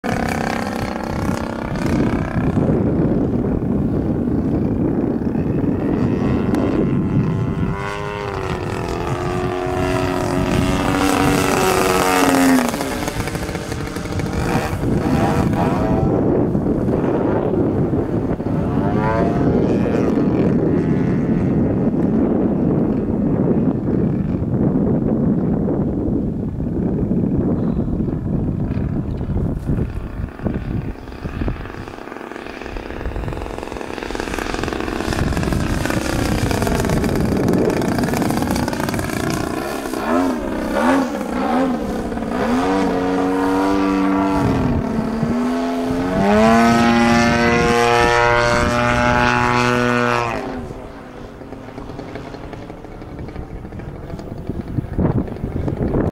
Flight testing prototype CRRC Pro GP50R
Flight testing a prototype CRRC Pro GP50r. JC 23 X 7 prop. Little rich on low end. 7450 rpm on top end.